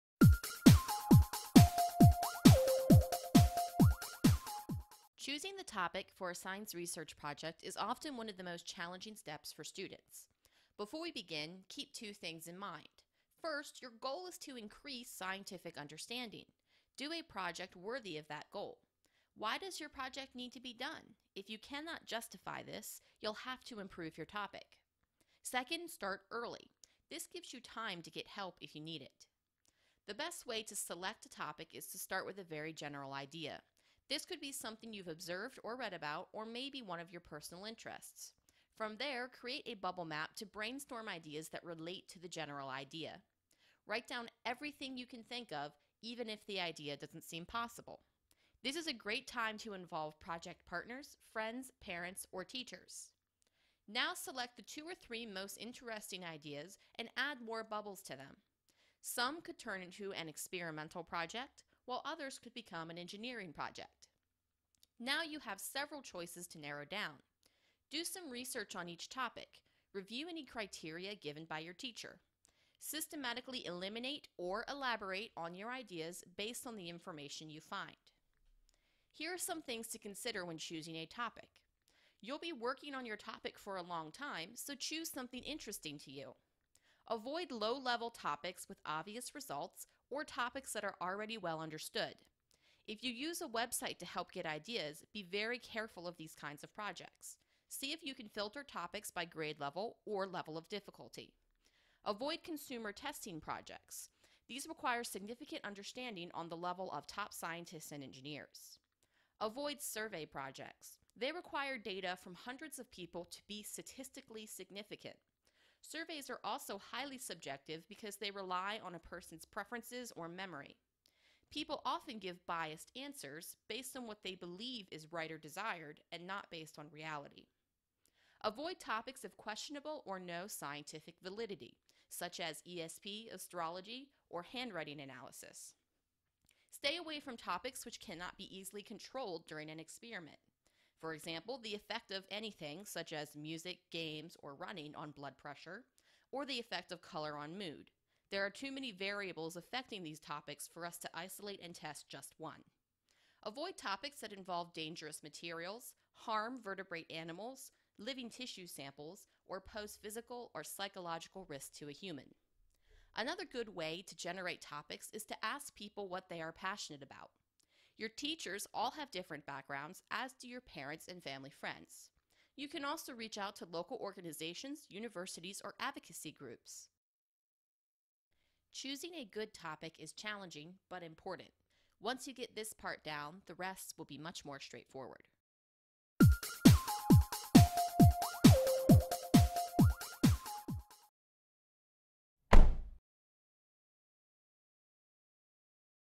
Select a Topic of Research
How do you select a topic for your science research project? Let's review some tips.